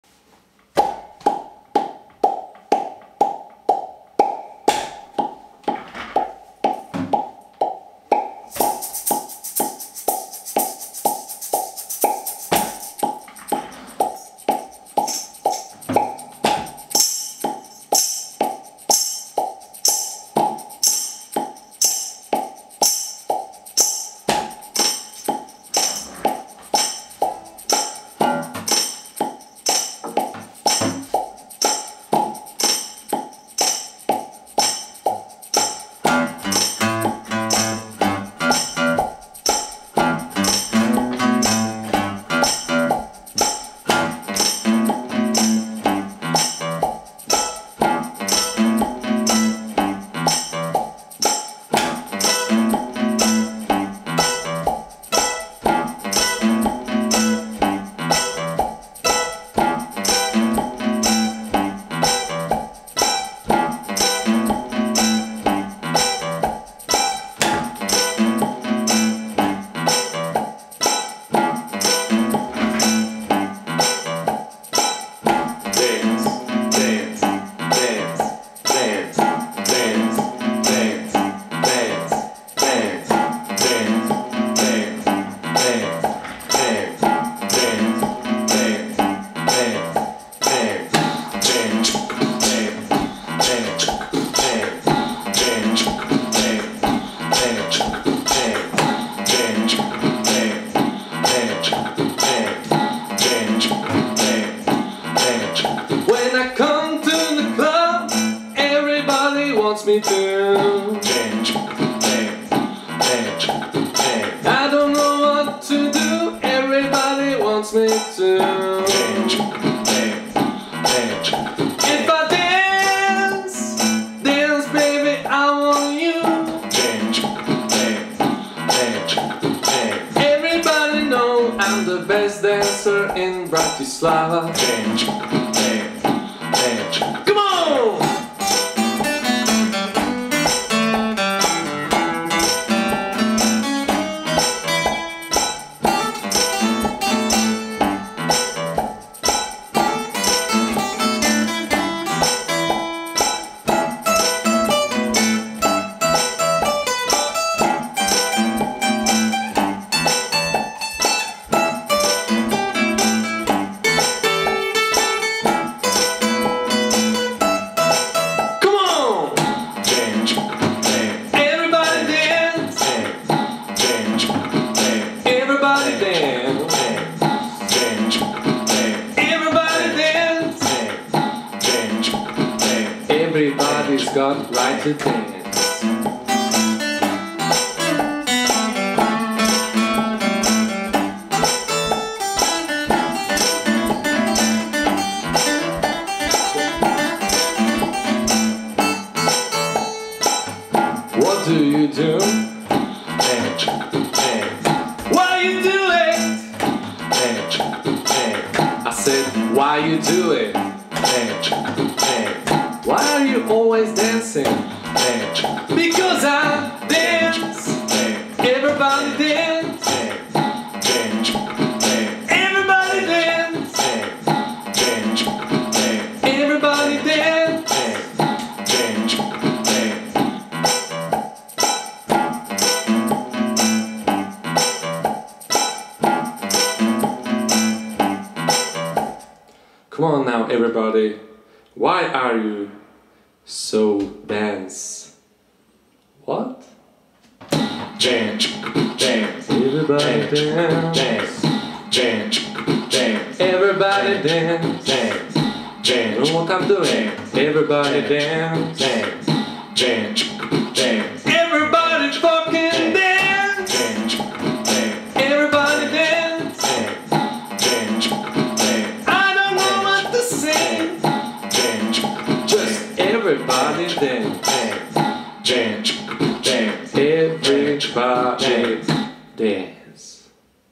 Loop 1 - Everybody dance
Do hudbnej výbavy pridávam loopstation Boos RC-30 a skúšam, čo dokáže. Myslím, že si s ním ešte užijem veľa zábavy.